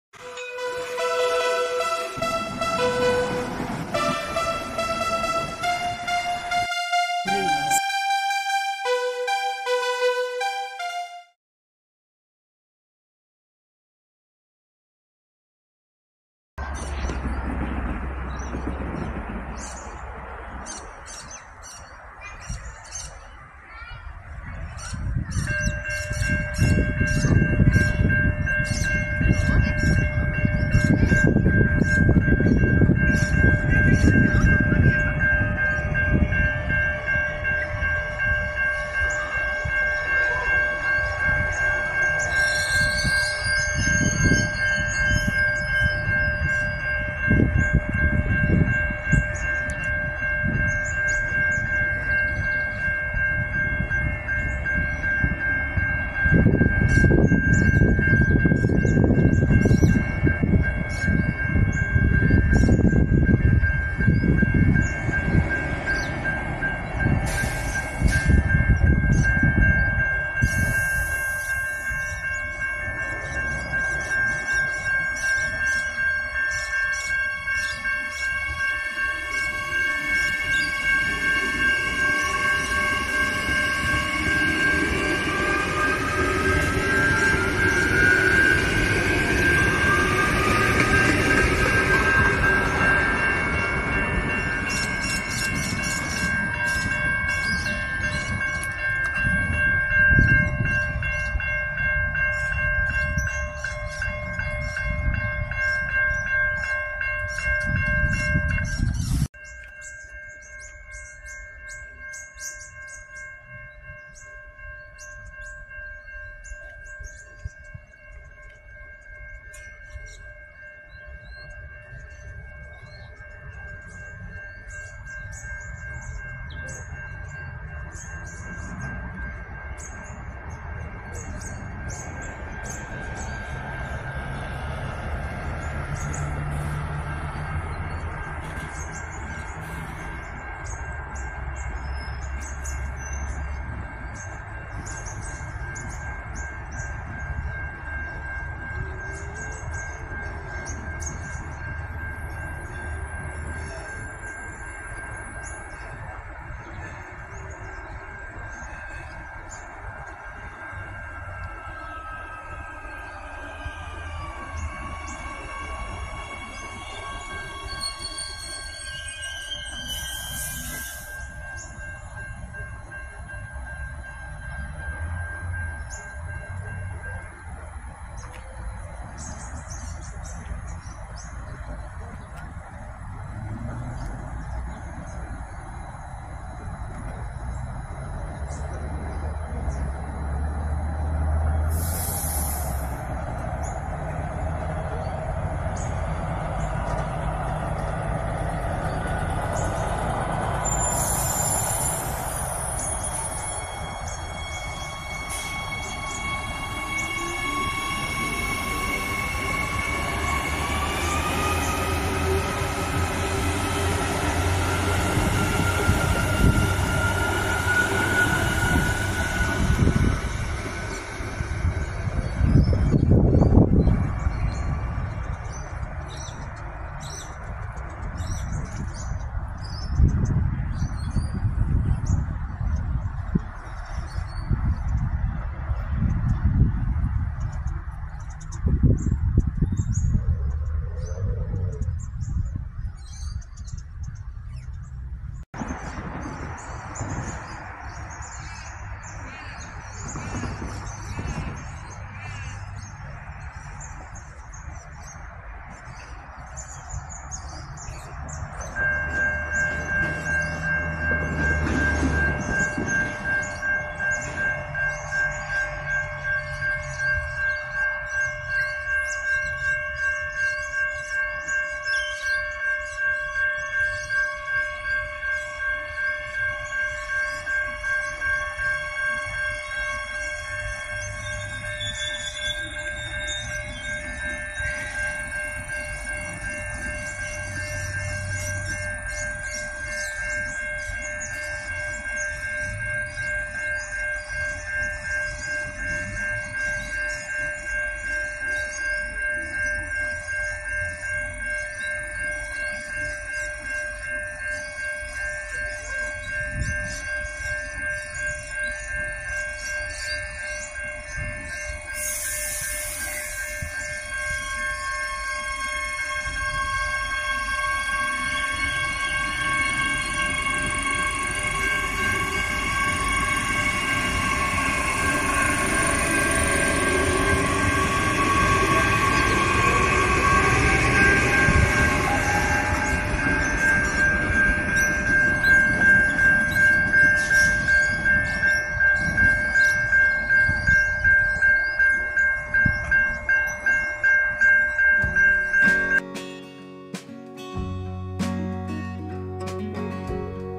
Some Videos of Trains at Salisbury.
The Trains in the Video are 4000 class trains that are in Adelaide.